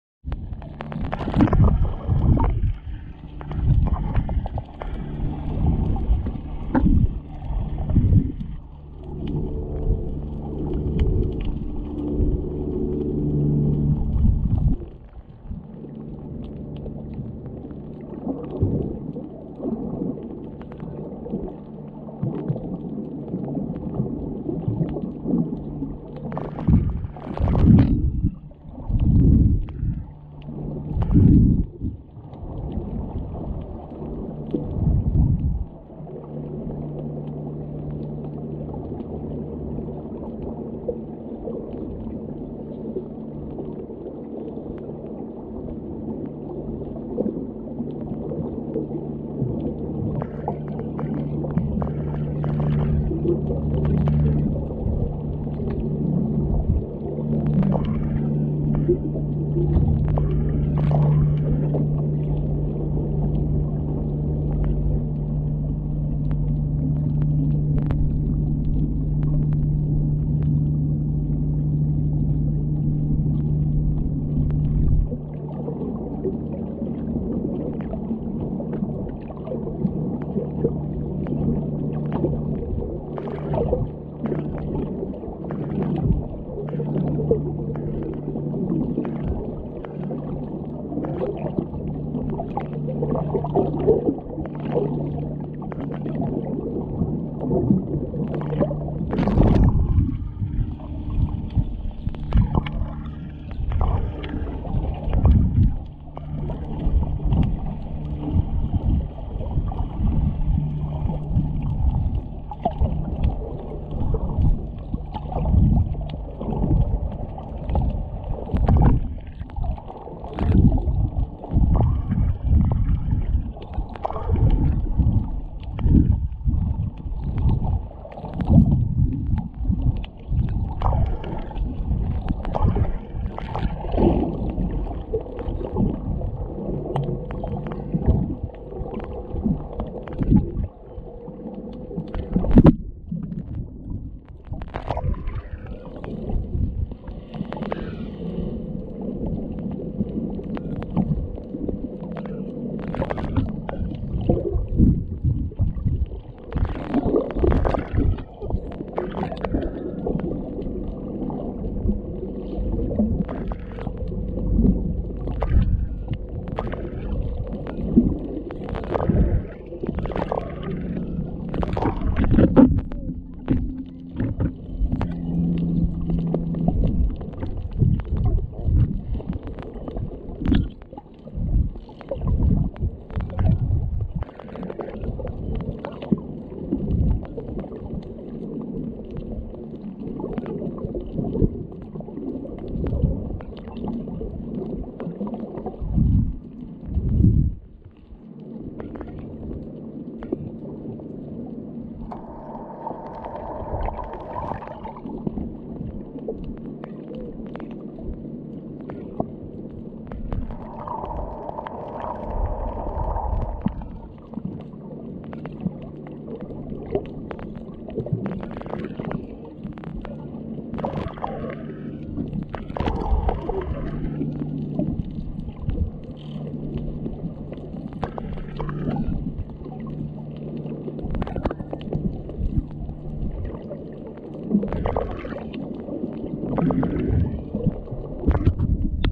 Wally the Wrasse and the grumpy GT
Snorkeling Great Barrier reef dec 2019 learning to use GoPro hero7